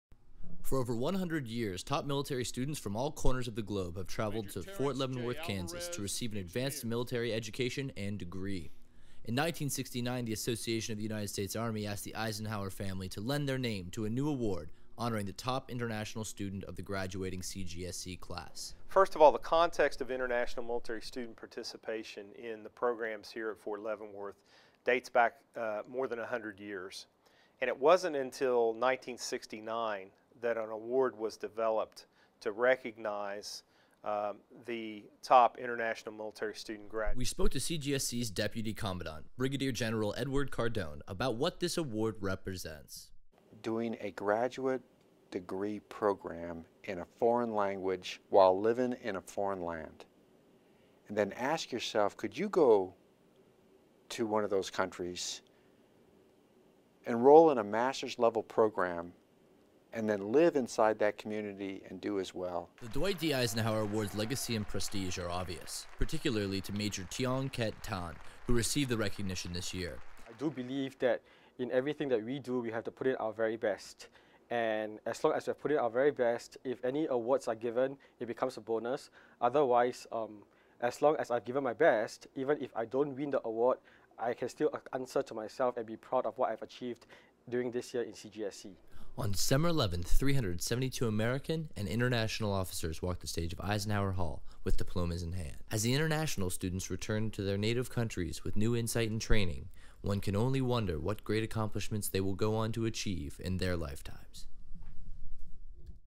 CGSC Eisenhower Award Winner 11DEC09
This is a Video News Release on the historic Eisenhower Award. On December 11, 2009 MAJ Tiong Keat Tan of Singapore received the prestigious honor. The award itself dates back to 1969, but international students have participated in the educational program at the Command and General Staff College in Fort Leavenworth for over 100 years.